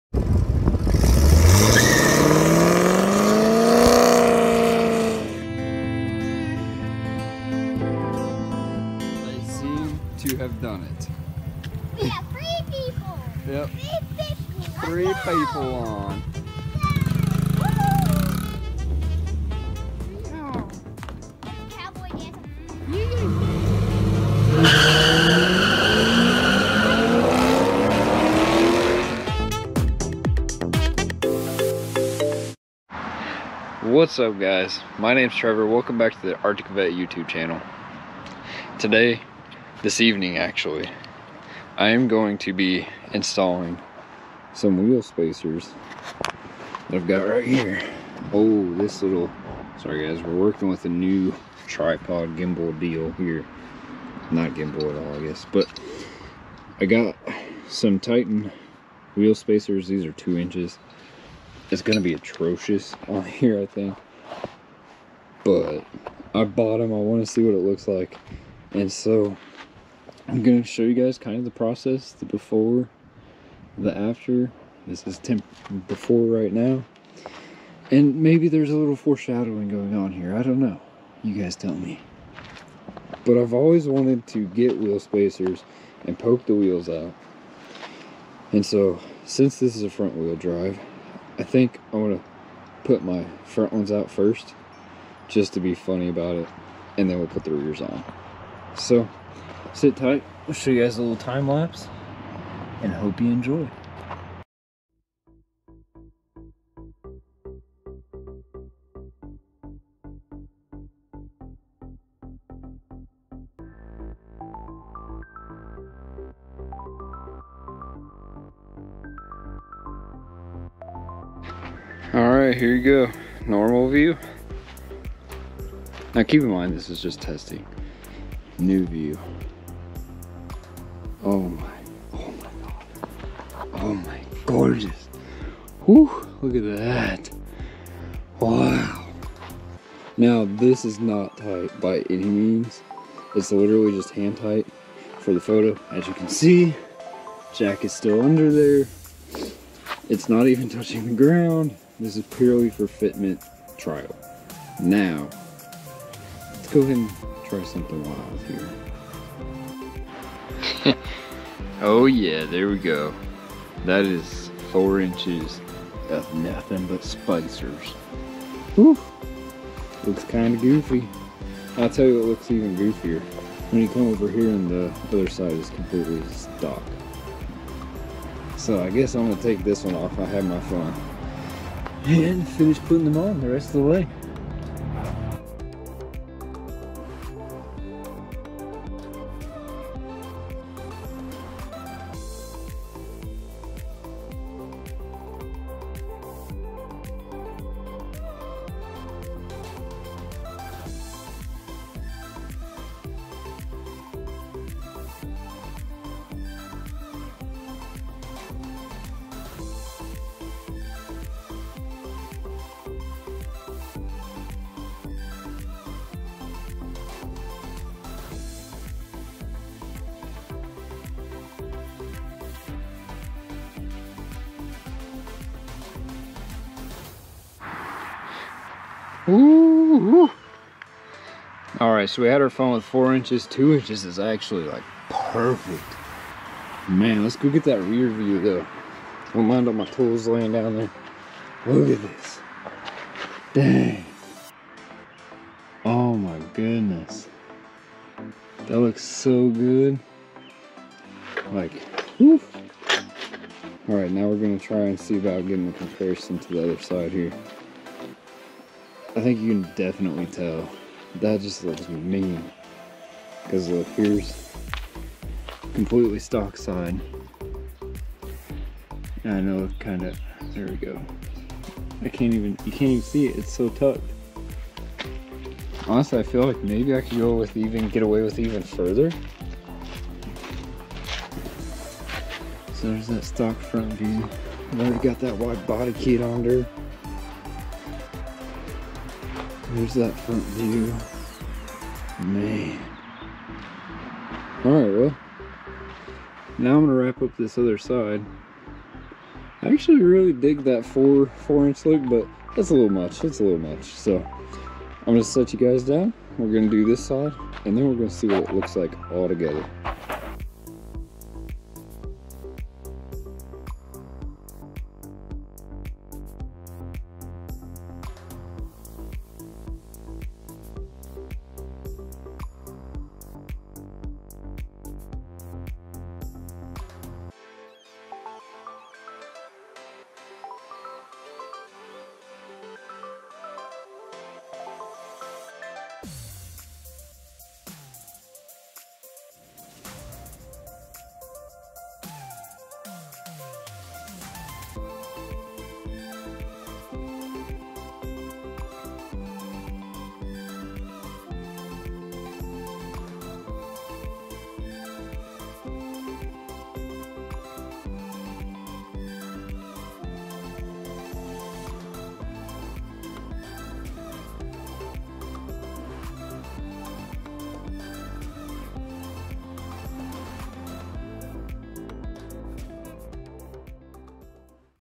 Adding Wheel Spacers to DORA teh Explorer! (2016 Ford Explorer 2" spacers)
Wheel Spacers

32gb SD Card (2 pack)